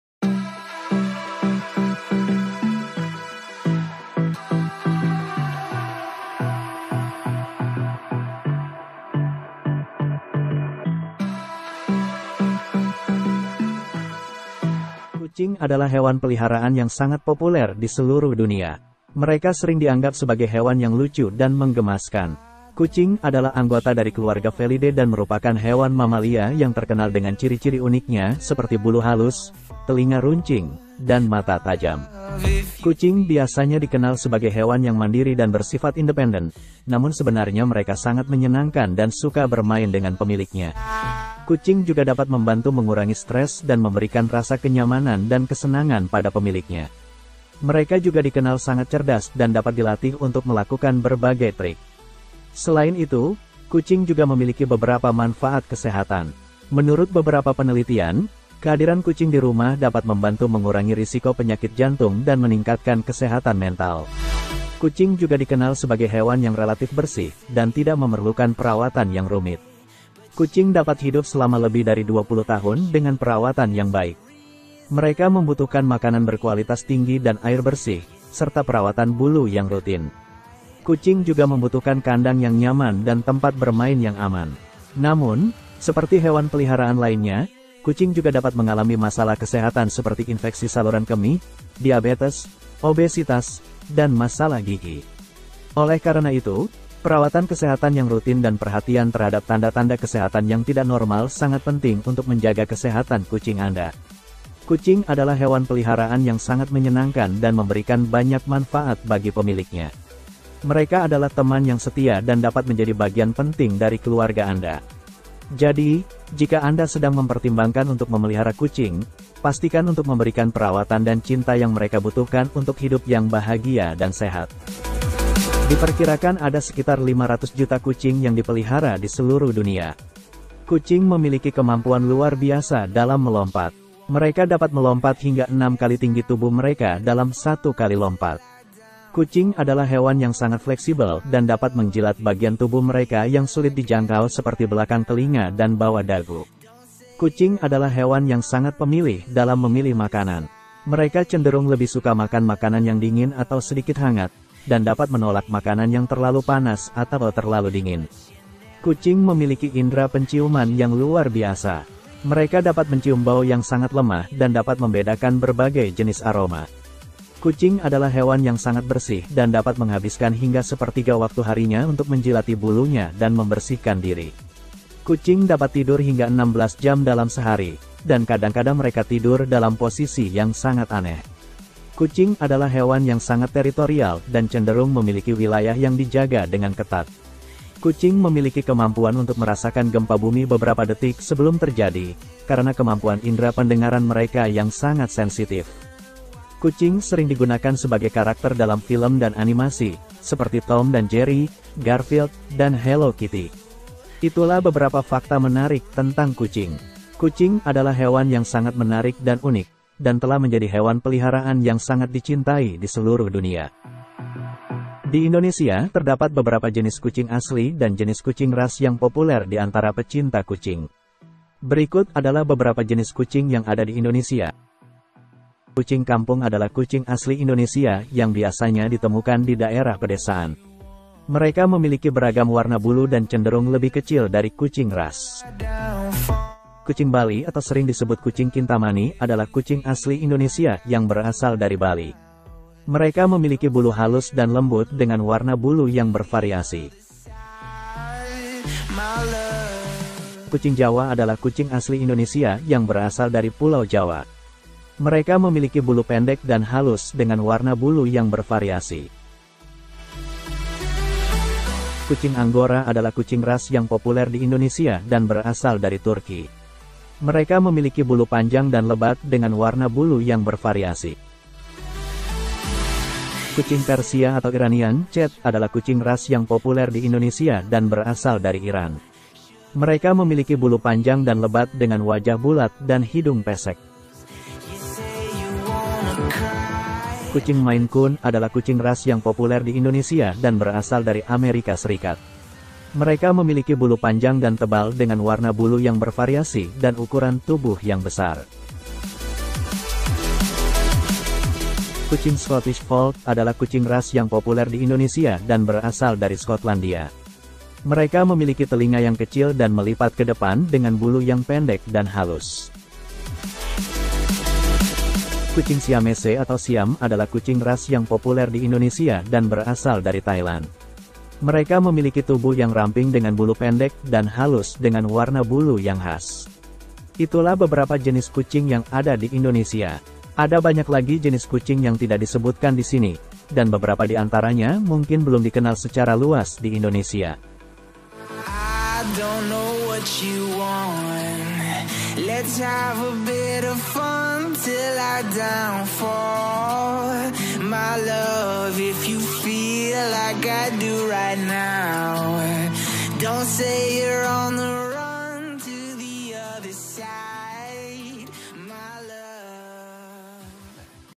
inilah beberapa jenis kucing di indonesia
Kucing adalah hewan peliharaan yang sangat populer di seluruh dunia mereka sering dianggap sebagai hewan yang lucu dan menggemaskan kucing adalah anggota dari keluarga felidae dan merupakan hewan mamalia yang terkenal dengan ciri-ciri uniknya seperti bulu halus telinga runcing dan mata tajam kucing biasanya dikenal sebagai hewan yang mandiri dan bersifat independen namun sebenarnya mereka sangat menyenangkan dan suka bermain dengan pemiliknya kucing juga dapat membantu mengurangi stres dan memberikan rasa kenyamanan dan kesenangan pada pemiliknya mereka juga dikenal sangat cerdas dan dapat dilatih untuk melakukan berbagai trik Selain itu kucing juga memiliki beberapa manfaat kesehatan menurut beberapa penelitian kehadiran kucing di rumah dapat membantu mengurangi risiko penyakit jantung dan meningkatkan kesehatan mental kucing juga dikenal sebagai hewan yang relatif bersih dan tidak memerlukan perawatan yang rumit kucing dapat hidup selama lebih dari 20 tahun dengan perawatan yang baik mereka membutuhkan makanan berkualitas tinggi dan air bersih serta perawatan bulu yang rutin kucing juga membutuhkan kandang yang nyaman dan tempat bermain yang aman namun seperti hewan peliharaan lainnya kucing juga dapat mengalami masalah kesehatan seperti infeksi saluran kemih diabetes obesitas dan masalah gigi Oleh karena itu perawatan kesehatan yang rutin dan perhatian terhadap tanda-tanda kesehatan yang tidak normal sangat penting untuk menjaga kesehatan kucing Anda Kucing adalah hewan peliharaan yang sangat menyenangkan dan memberikan banyak manfaat bagi pemiliknya Mereka adalah teman yang setia dan dapat menjadi bagian penting dari keluarga anda Jadi jika anda sedang mempertimbangkan untuk memelihara kucing pastikan untuk memberikan perawatan dan cinta yang mereka butuhkan untuk hidup yang bahagia dan sehat diperkirakan ada sekitar 500 juta kucing yang dipelihara di seluruh dunia kucing memiliki kemampuan luar biasa dalam melompat mereka dapat melompat hingga 6 kali tinggi tubuh Mereka dalam satu kali lompat Kucing adalah hewan yang sangat fleksibel dan dapat menjilat bagian tubuh mereka yang sulit dijangkau seperti belakang telinga dan bawah dagu Kucing adalah hewan yang sangat pemilih dalam memilih makanan Mereka cenderung lebih suka makan makanan yang dingin atau sedikit hangat dan dapat menolak makanan yang terlalu panas atau terlalu dingin kucing memiliki indra penciuman yang luar biasa mereka dapat mencium bau yang sangat lemah dan dapat membedakan berbagai jenis aroma Kucing adalah hewan yang sangat bersih dan dapat menghabiskan hingga sepertiga waktu harinya untuk menjilati bulunya dan membersihkan diri kucing dapat tidur hingga 16 jam dalam sehari dan kadang-kadang mereka tidur dalam posisi yang sangat aneh Kucing adalah hewan yang sangat teritorial dan cenderung memiliki wilayah yang dijaga dengan ketat kucing memiliki kemampuan untuk merasakan gempa bumi beberapa detik sebelum terjadi karena kemampuan indera pendengaran mereka yang sangat sensitif kucing sering digunakan sebagai karakter dalam film dan animasi seperti Tom dan Jerry Garfield dan Hello Kitty itulah beberapa fakta menarik tentang kucing Kucing adalah hewan yang sangat menarik dan unik dan telah menjadi hewan peliharaan yang sangat dicintai di seluruh dunia di Indonesia terdapat beberapa jenis kucing asli dan jenis kucing ras yang populer di antara pecinta kucing.